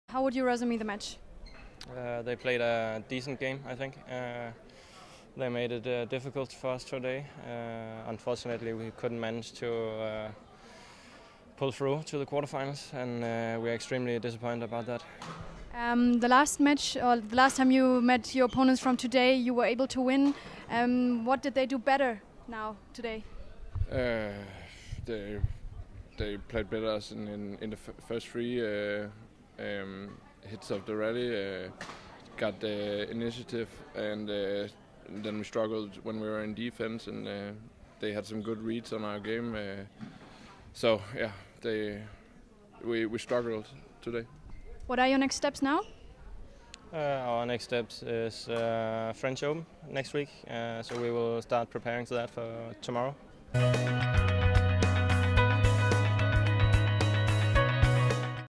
"We struggled today"
Kim Astrup and Anders Skaarup Rasmussen after losing to Liao Min Chun and Su Ching Heng from Chinese Taipei in the round of 16 at the DANISA Denmark Open